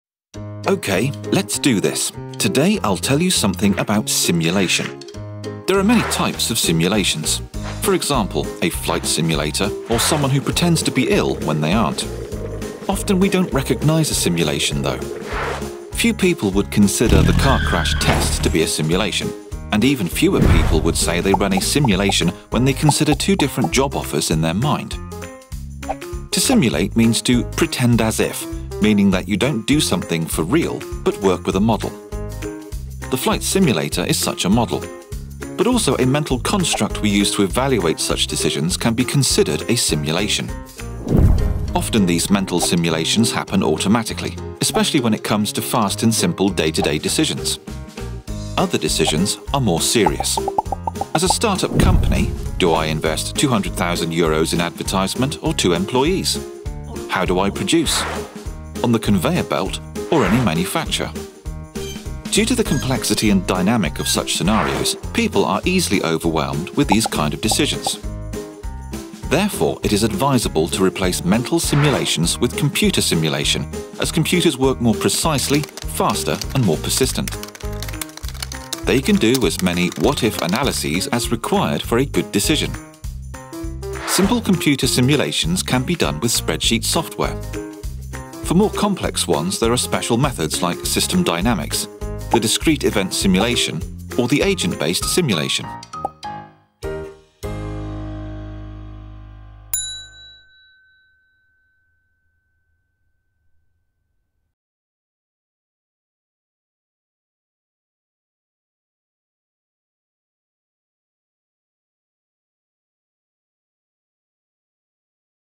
Application of computer simulation in business
You want to dive deep into the world of finance and management? Visit us:

There are many types of simulations - for example a flight simulator or someone who
pretends to be ill when they aren’t. Often we don’t recognise a simulation, though. Few people would consider the car crash test to be a simulation. And even fewer people would say they run a simulation when they consider two different job offers in their mind. To simulate means to “pretend as if” meaning that you don’t do something for real but work with a model. The flight simulator is such a model.
But also a construct we use to evaluate such decisions can be considered a simulation. Often these mental simulations happen automatically. Especially when it comes to fast and simple day-to-day decisions. Other decisions are more serious.
As a start-up company, do I invest 200.000 € in advertisement or two employees?
How do I produce? On the conveyor belt or in a manufacture?
Due to the complexity and dynamic of such scenarios people are easily overwhelmed with these kind of decisions. Therefore it is advisable to replace mental simulations with computer simulation as computers work more precisely, faster and more persistent. They can do as many “what-if-analyses” as required for a good decision.
Simple computer simulations can be done with table calculation software. For more
complex ones there are special methods like “system dynamics”, the discrete event
simulation or the agent based simulation.